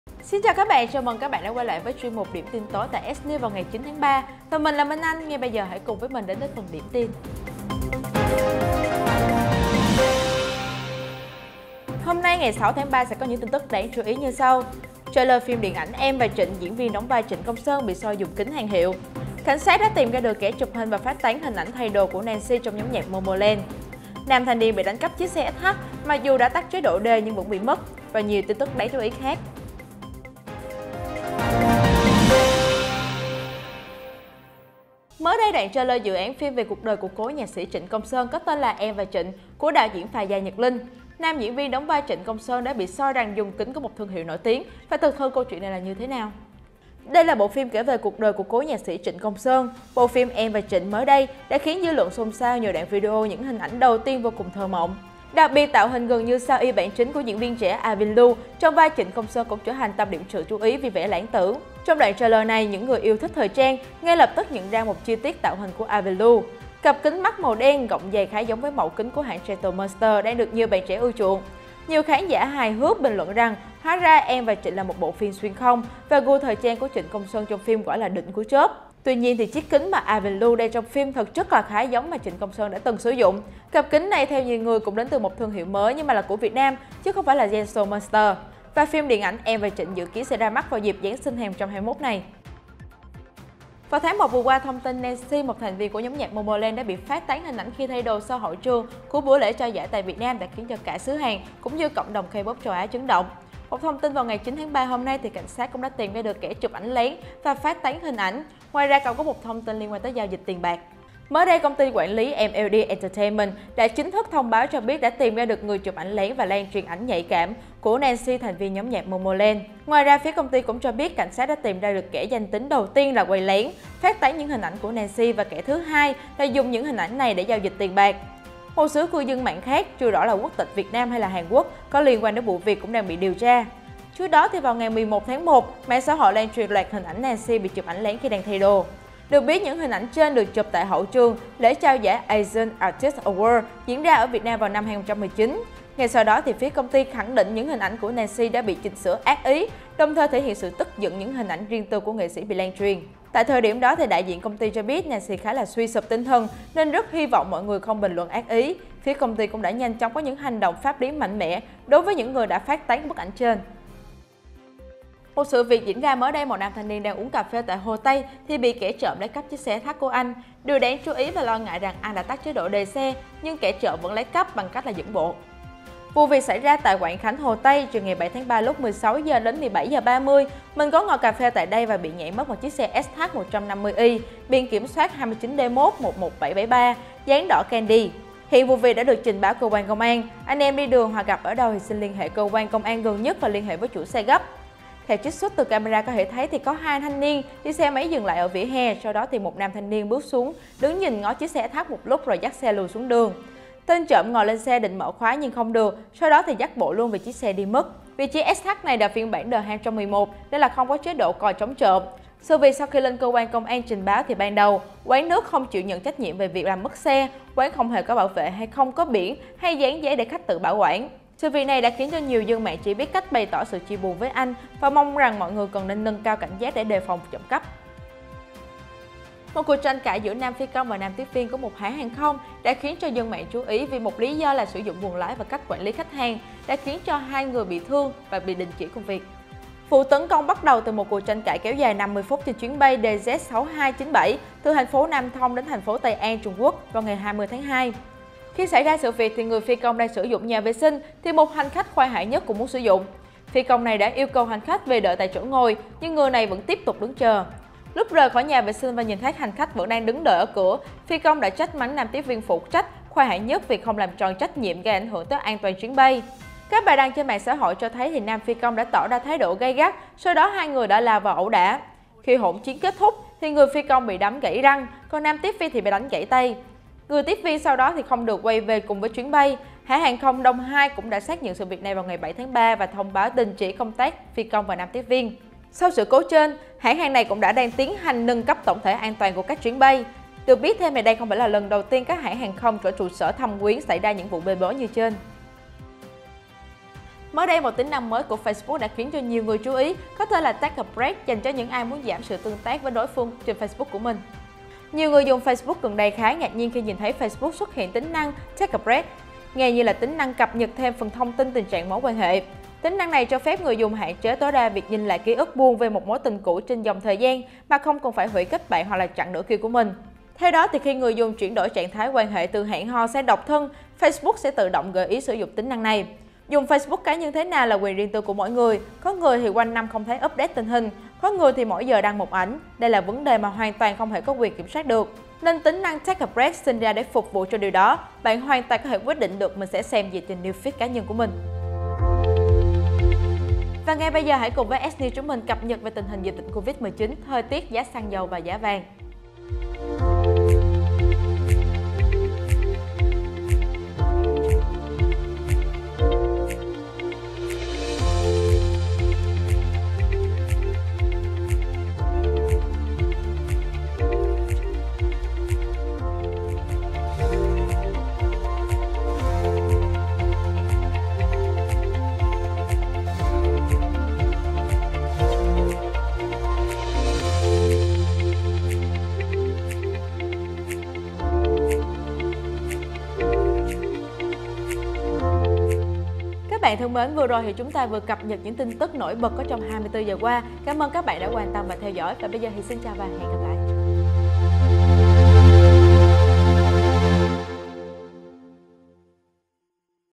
Trịnh Công Sơn bị soi dùng kính hàng hiệu trong trailer; Cảnh sát tìm ra kẻ chụp lén nữ idol Nancy
ĐIỂM TIN TỐI 09/03/2021 gồm những tin chính sau:

00:00 Chào đầu bản tin
00:35 Trịnh Công Sơn bị soi dùng kính hiệu chất đét trong trailer, thực hư ra sao?
01:33 Cảnh sát tìm ra kẻ chụp lén ảnh nhạy cảm của nữ idol Nancy ở AAA Việt Nam
02:45 Ngồi uống cafe ở Hồ Tây, thanh niên bị kẻ trộm lấy mất chiếc SH trăm triệu
03:49 Tranh cãi việc sử dụng buồng vệ sinh, phi công và tiếp viên lao đánh nhau trên máy bay
04:56 Facebook có "Take a break", dành cho những ai 'Bằng mặt không bằng lòng'
05:43 TIN TỨC VỀ DỊCH COVID-19, DỰ BÁO THỜI TIẾT, GIÁ XĂNG DẦU, GIÁ VÀNG